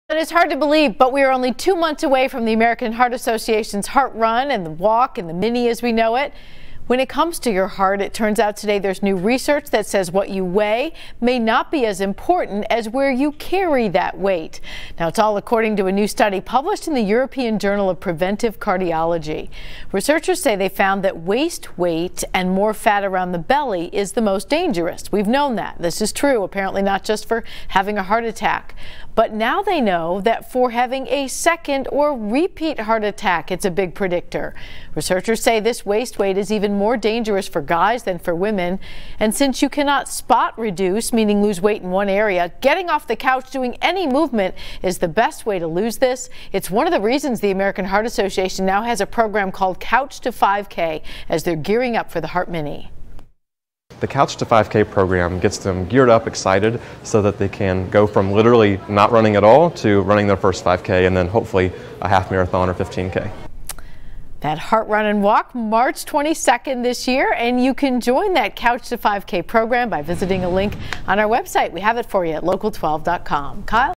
Study: Belly fat is risk for repeat heart attacks
CINCINNATI (WKRC) - When it comes to your heart, what you weigh may not be as important as where you carry that weight.

This is according to a new study published in the European Journal of Preventive Cardiology. Researchers found that waist weight and more fat around the stomach is the most dangerous.

This is true apparently not just for having a heart attack, but now for having a second or repeat heart attack. Researchers say this waist weight is even more dangerous for men than women.

You can't spot reduce, but getting off the couch and doing any movement helps.

You can get some movement by signing up for the American Heart Association's Heart Mini.